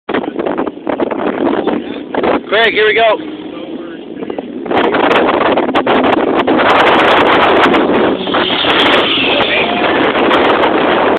Hotrails Sand Rail Jumping Choke Cherry in St. Anthonys Idaho
Jumping my Hotrails sandcar off the top of Chokecherry in St. Anthony's Idaho